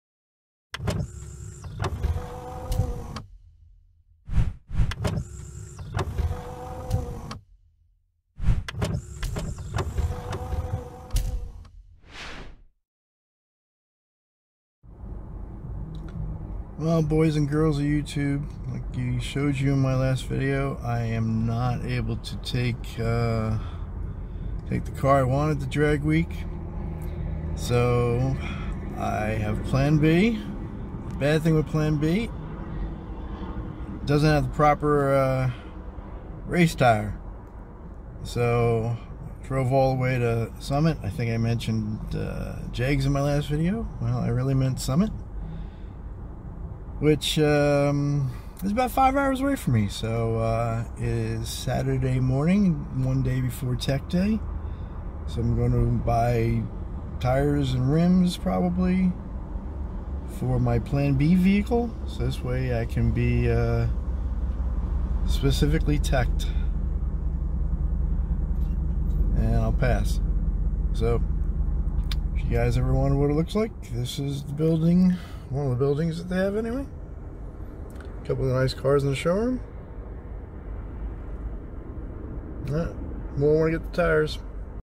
Drag Week 2019 - Getting Tires for plan B
Well this is not what I wanted to do for Drag Week 2019 but I was not going to miss participating again. So after a 5 hour drive and an overnight stay in McDonough, Georgia I'm at Summit to buy drag radials for a 2014 Ford Fiesta. Side note: Mickey Thompson ET Street 205/50 15 fit really well. Anyone want to buy a set slightly used?